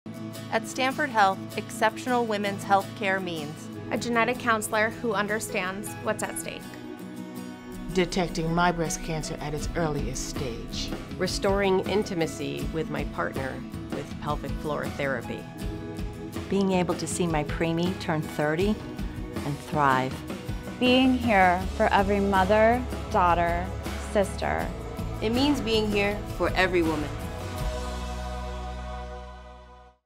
Stamford Health: Exceptional Women’s Health Care
At Stamford Health, exceptional women’s health care means expert, compassionate care for everything from the everyday to the complex. From gynecological care and pregnancy care to treatment for life-threatening cancers and illnesses, we're here for every woman in Fairfield County.